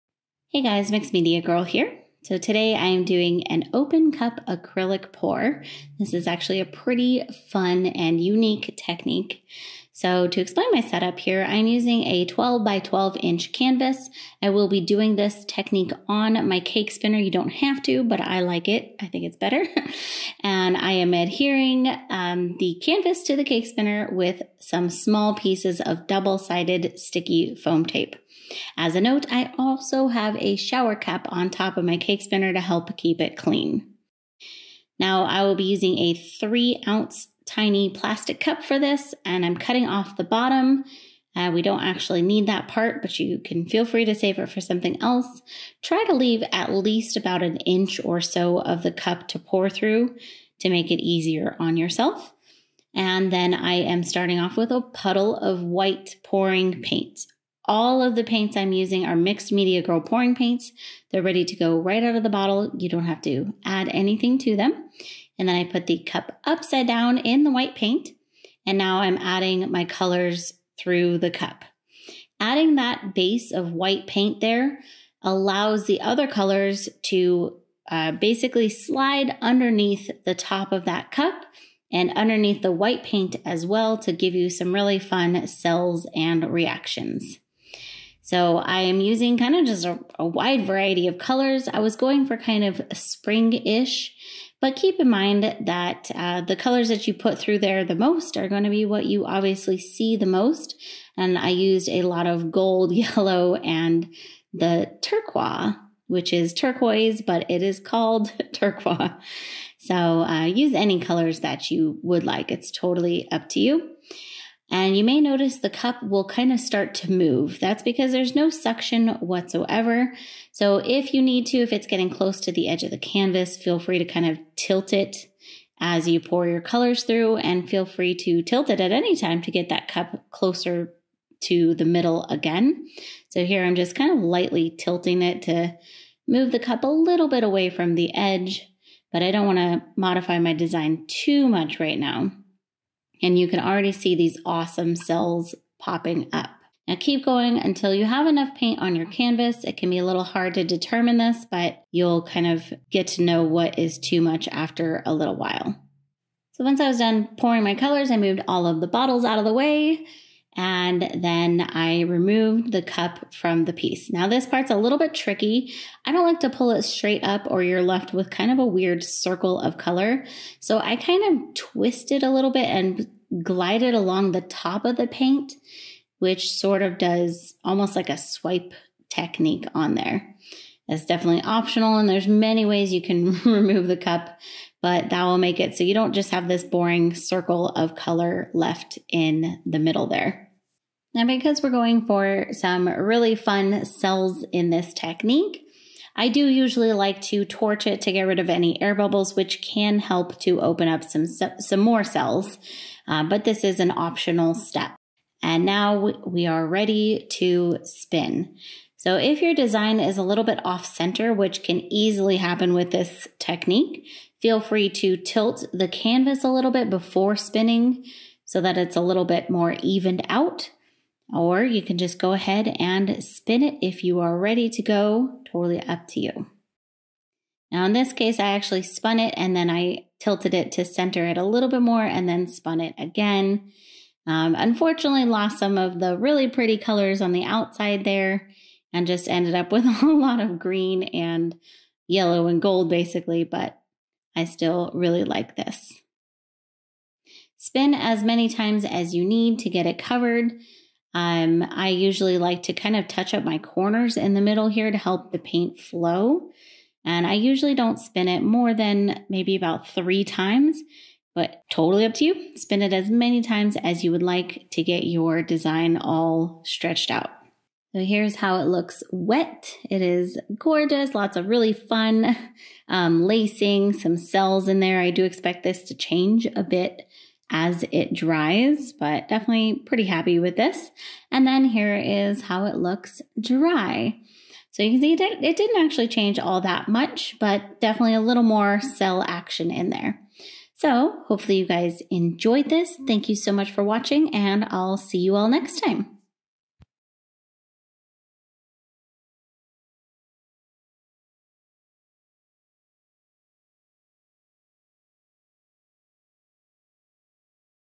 Open Cup Acrylic Pour on a Cake Spinner!
Open cup acrylic pours are so fun! Some of my colors disappeared and that was a little sad, but I still like it!

12x12 canvas
Cake Spinner